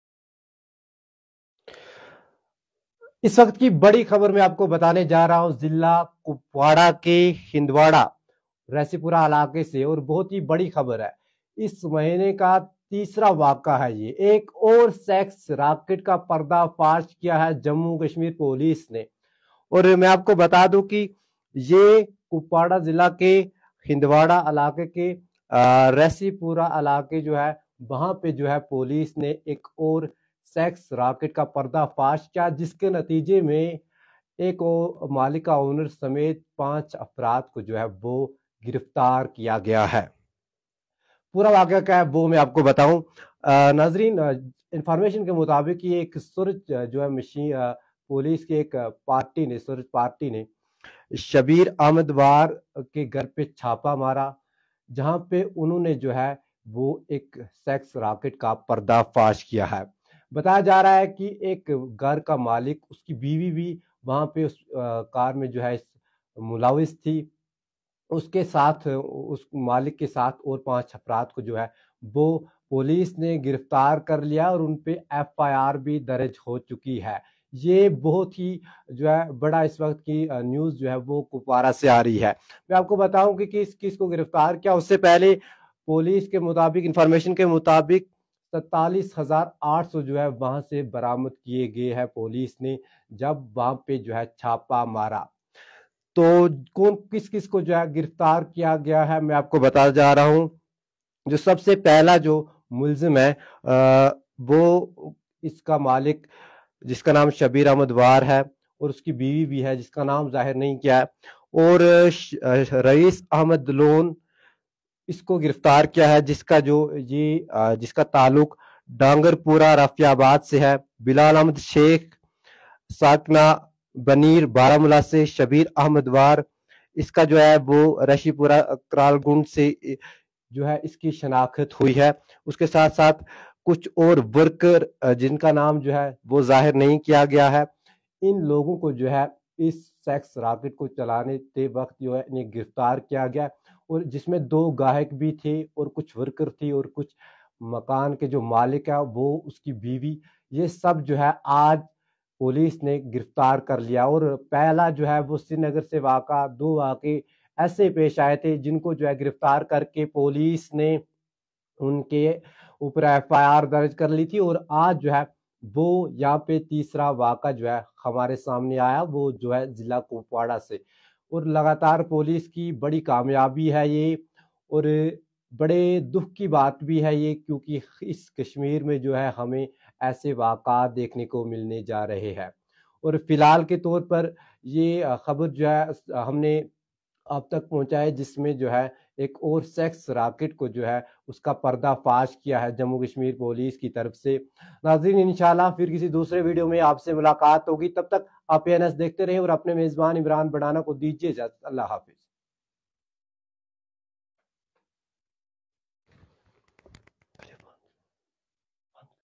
Shocking : Third sex racket busted in Kashmir in just a month
Now house owner, his wife, two men and a woman held in north Kashmir's Handwara.